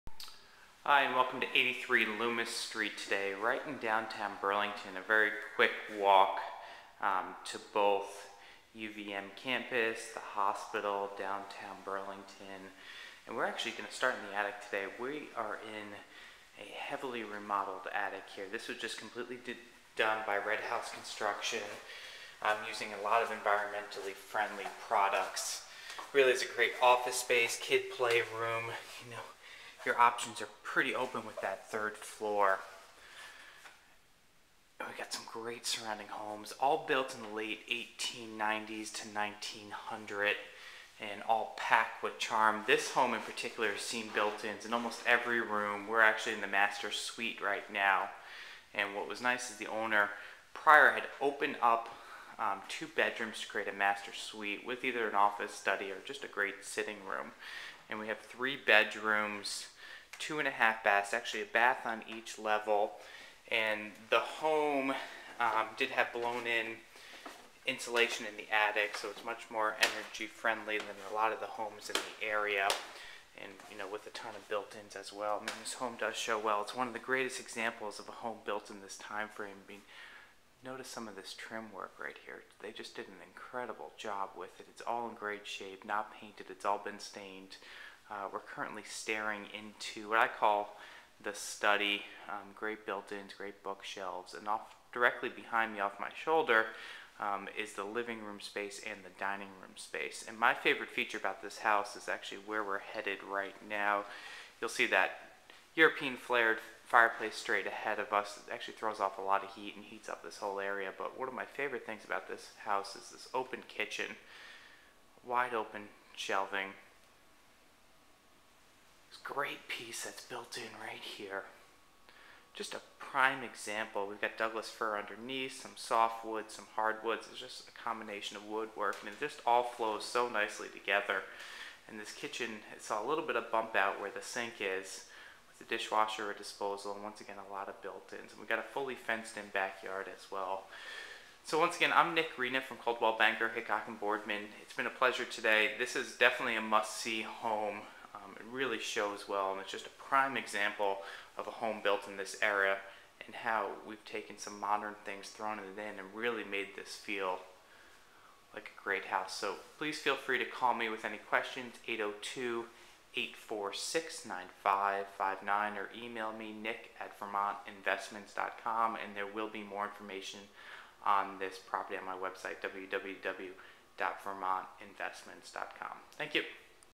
SOLD! 83 Loomis Street, Burlington Vermont
3 bedroom and 2 1/2 bathroom home in the heart of Burlington, VT.   Close to UVM and Fletcher Allen.